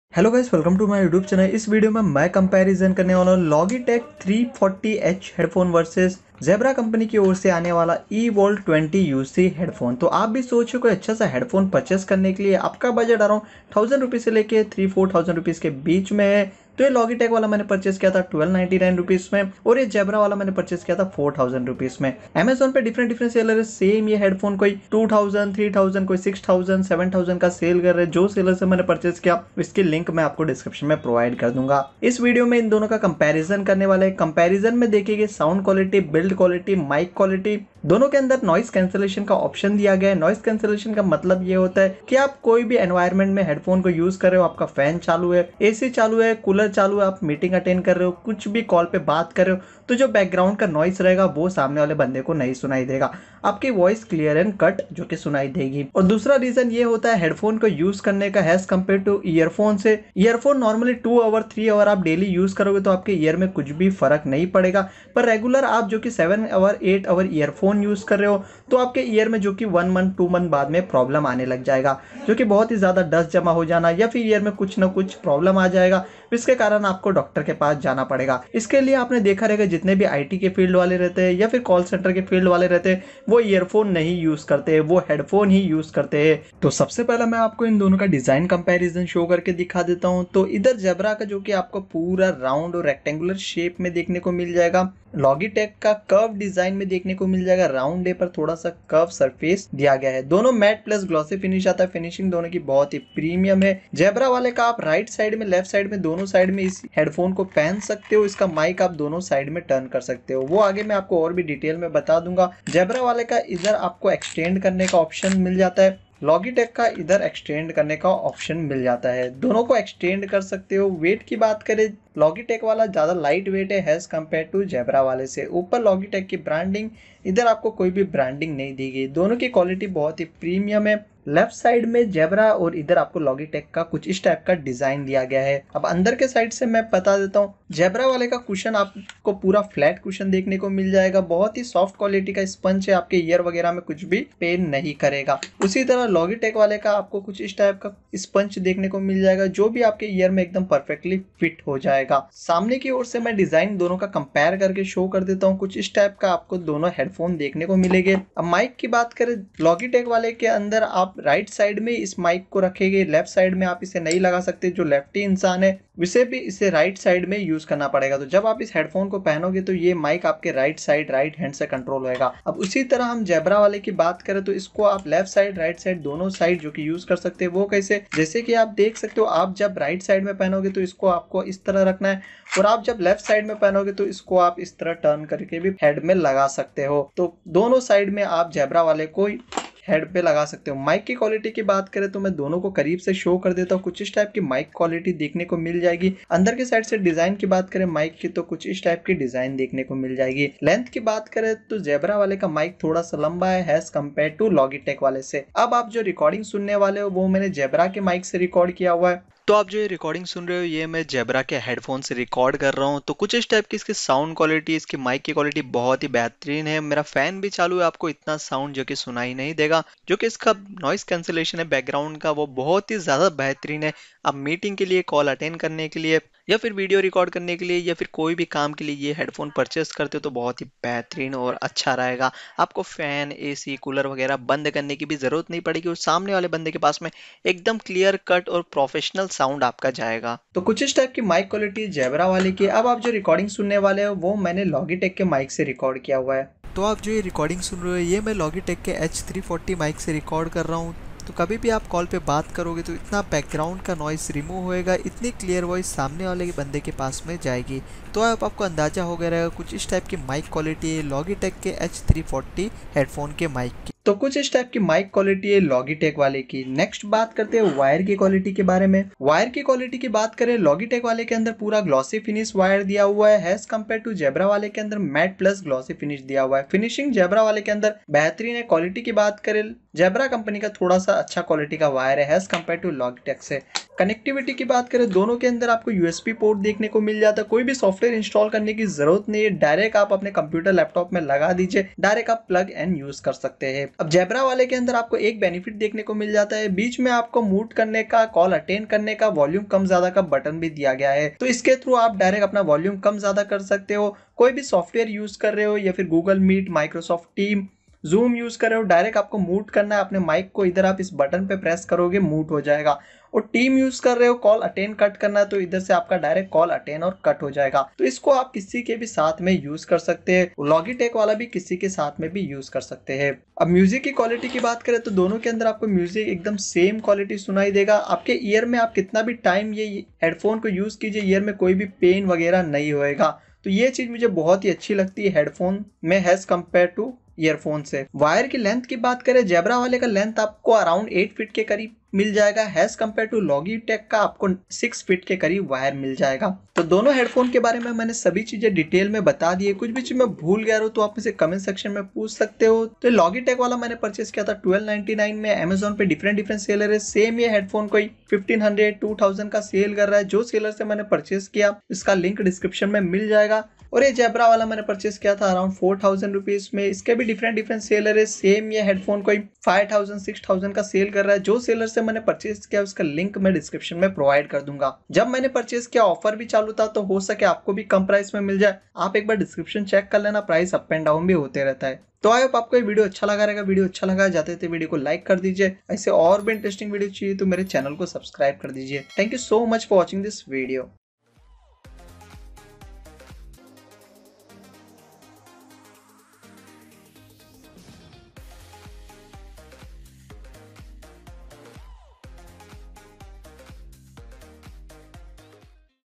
Logitech H340 headphone vs Jabra GN Netcom Evolve 20 UC Duo detail comparsion ( MS team, Meet) 2021
Jabra GN Netcom Evolve 20 UC Duo MS Optimized, USB Headband

Logitech H340 Wired USB Business On-Ear Stereo Headphones with Noise-Cancelling Microphone, Digital Stereo Sound on VOIP, Skype and webinars, PC/Mac/Laptop, Black

🖥️ Led Monitor
💻 Laptop which I use for Video editing
🎤Boya Microphone
🎤Another Blue Snowball iCE USB mic